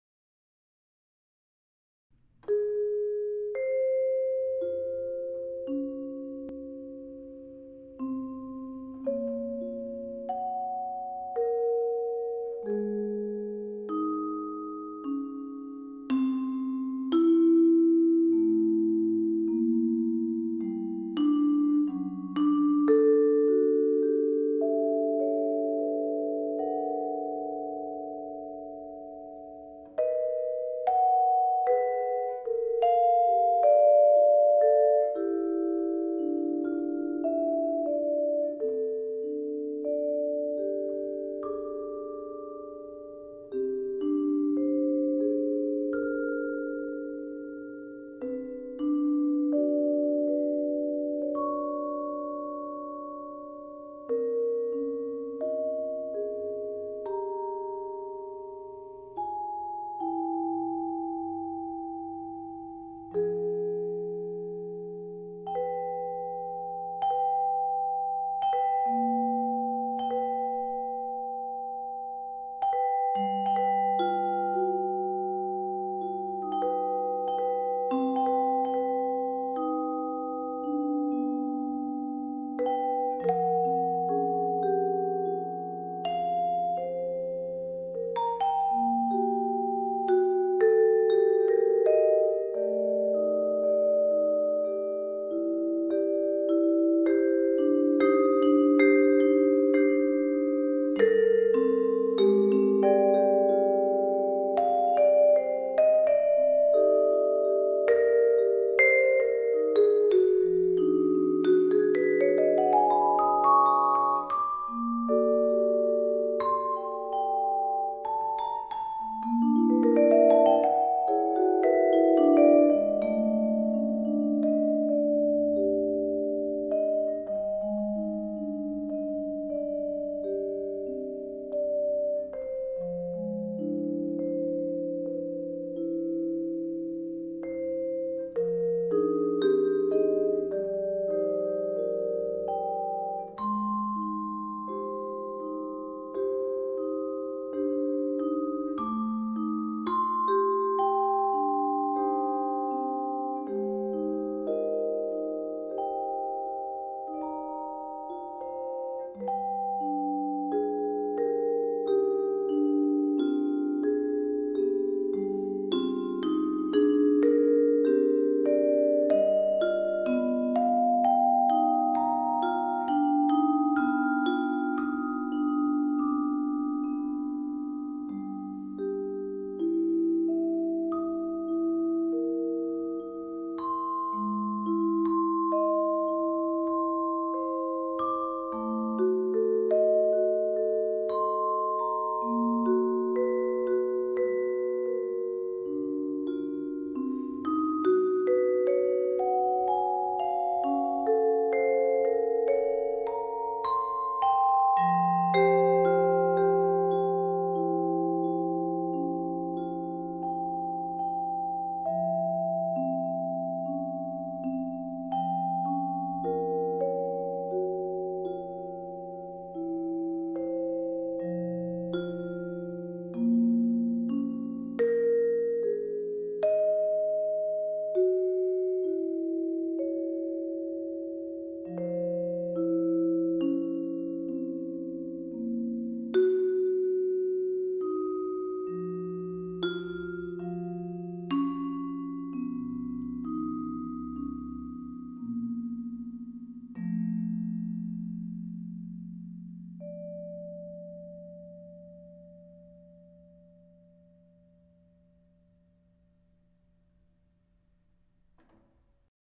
Daily Improvisation #365 - Vibraphone Solo (Julius Heise)
Welcome to my last (public) daily vibraphone improvisation!